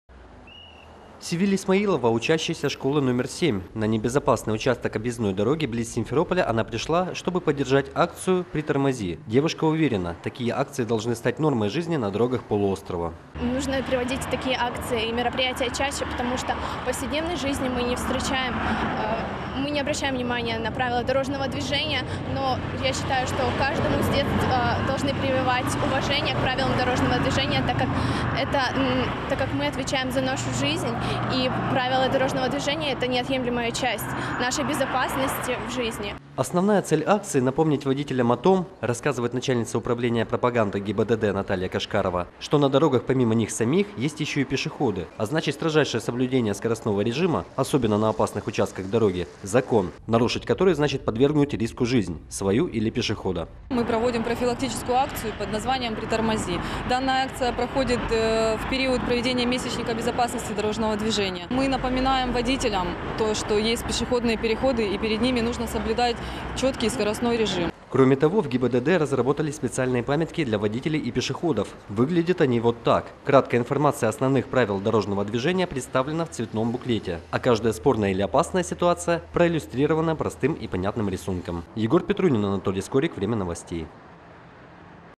В Крыму завершилась акция ГИБДД – «притормози»
В рамках месячника безопасности на дорогах, повсеместно на полуострове юные инспекторы дорожно-транспортного движения призывали водителей помнить о правилах дорожного движения и нормах техники безопасности. Молодые люди делали это с помощью собственноручно изготовленных транспарантов. Непосредственно в близи проезжей части.